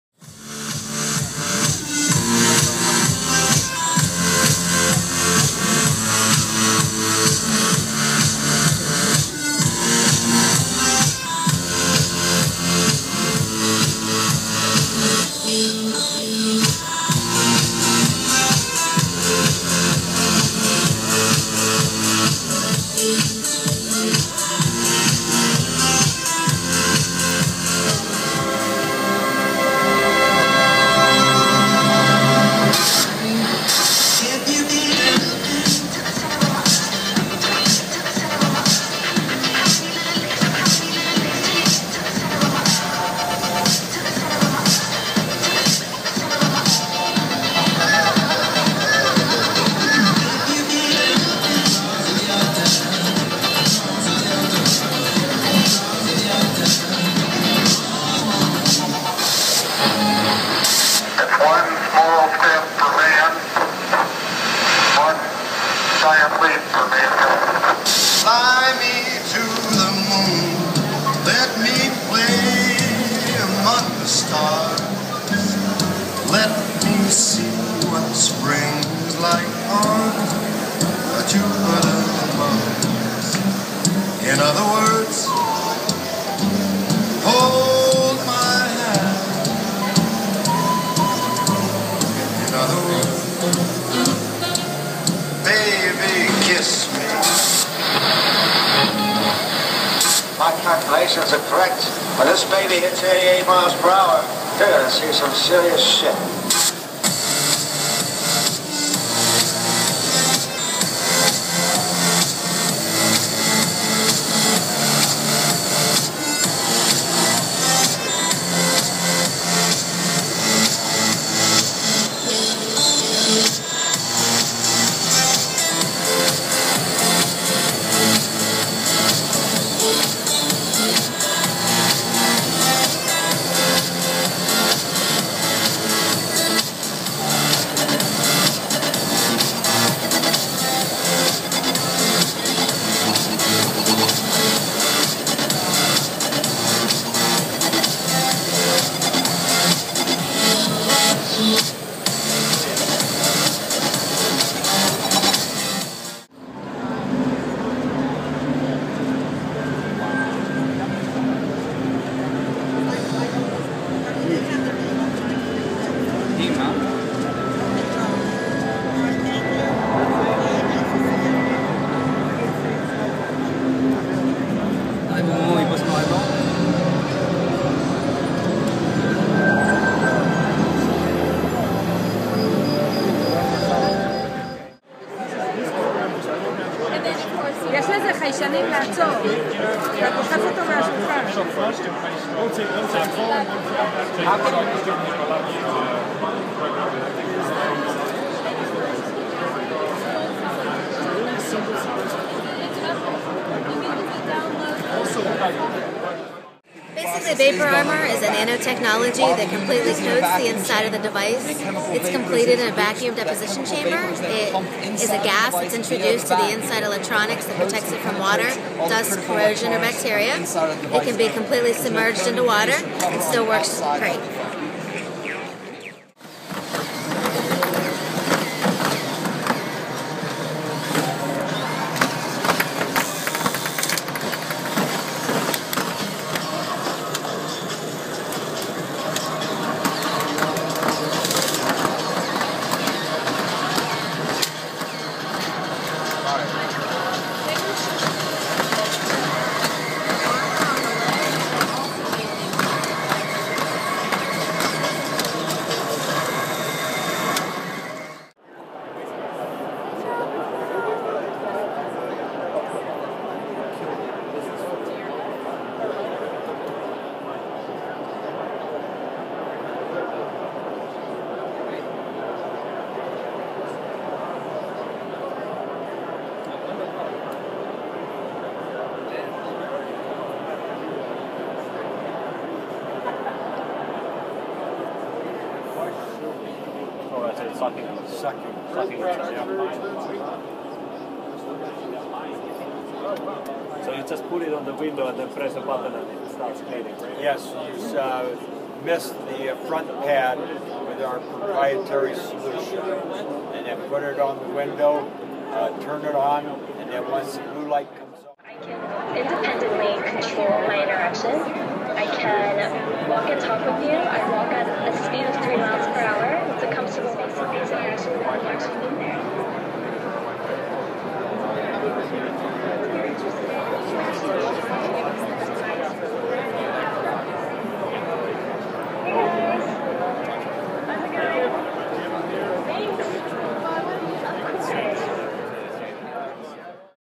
CES 2013
Clips from the CES exhibition in Las Vegas, January 2013